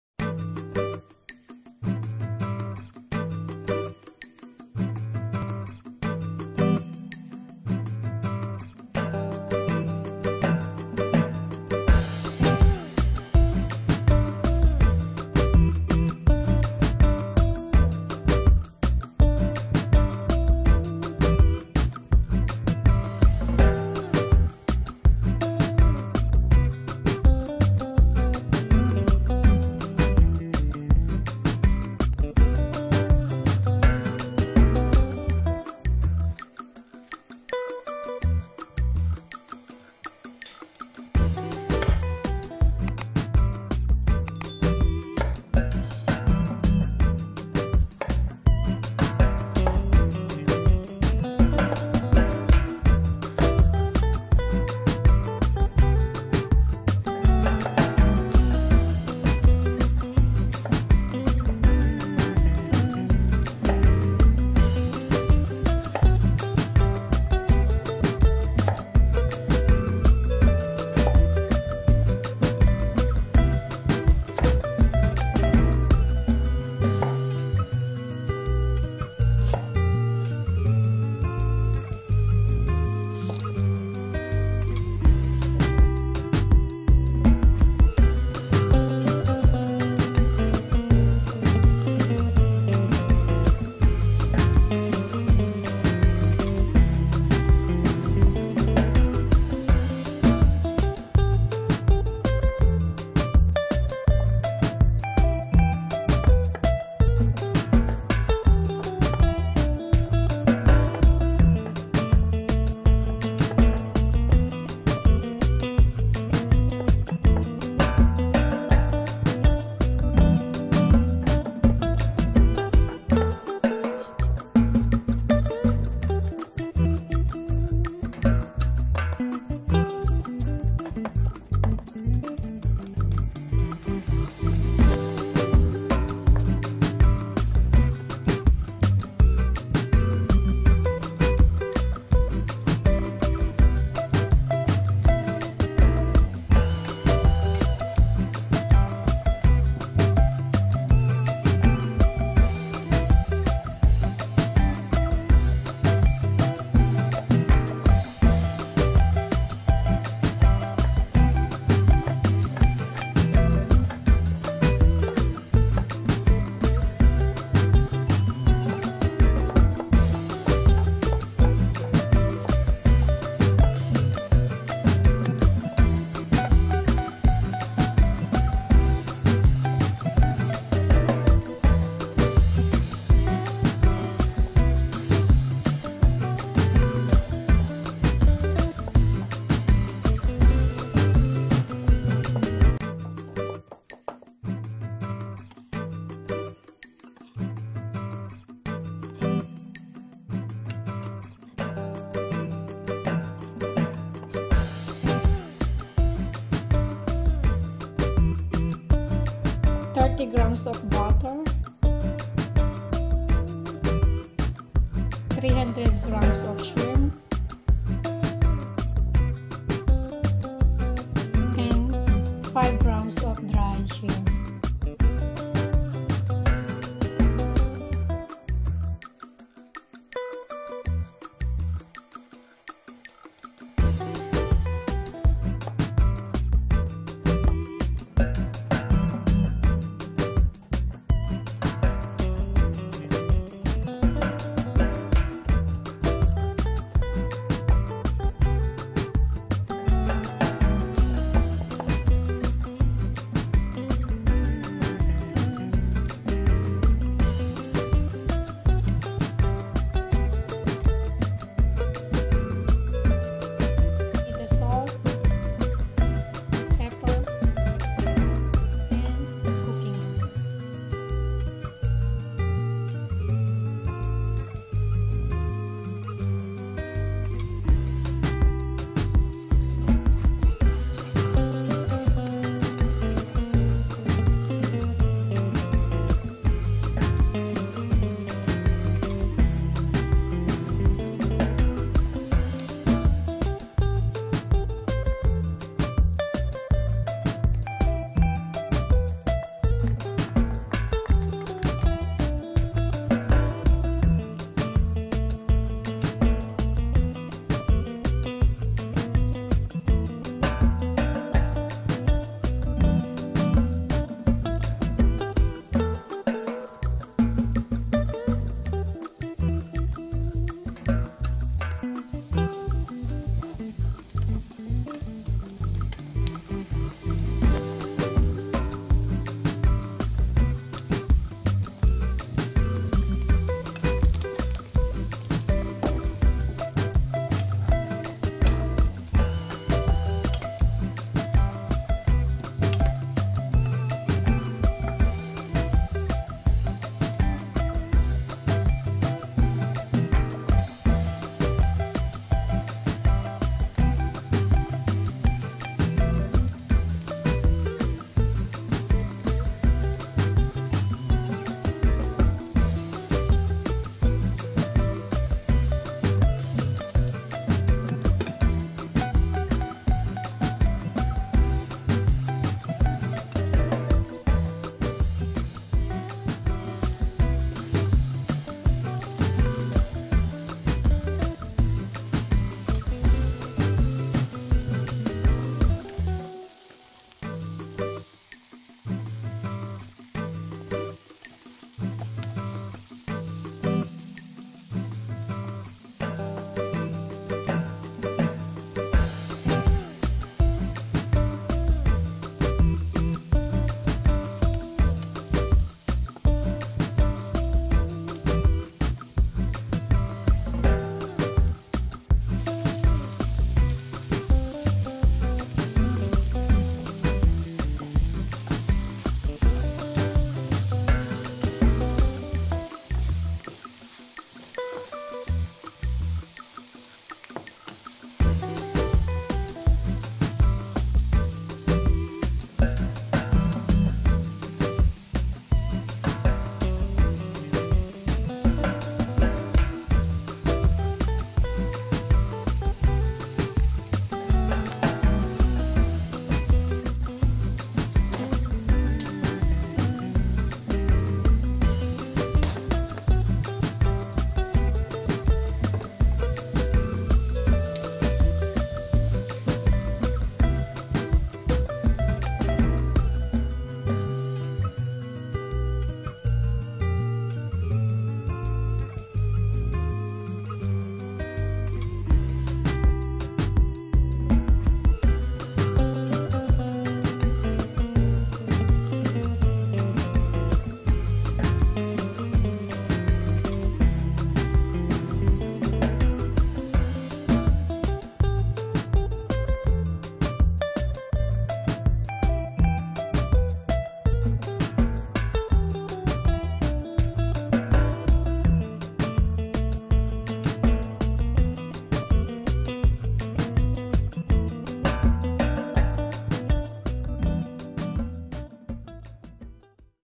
SAUTEED ZUCCHINI WITH SHIRMPS HOW TO COOK // MITCH IN WONDERLAND //
SAUTEED ZUCCHINI WITH SHRIMPS HOW TO COOK // MITCH IN WONDERLAND //

Ingredients:
1 big zucchini
1big onion
5 gloves garlic
salt
pepper
300 g shrimps
5 g dry salted shirmps
300 g unsalted butter

cut the zucchini into your desires cube size,sepereta after chopped,the next chop the five gloves of garlic and onion into square cube chopp...in other side preheat the pan and pour a few drops of cooking oil and start to sautee the garlic and onion until get golden brown.Add up the zucchini sautee and cover with the lid and simmer for about atleast five minutes and noted please don't overcook the zucchini vegetable.add up the shrimp and dry salted shirmp simmer atleast one or  two minutes cover with the lid,check time to time.

transfer to the serving plate and serve.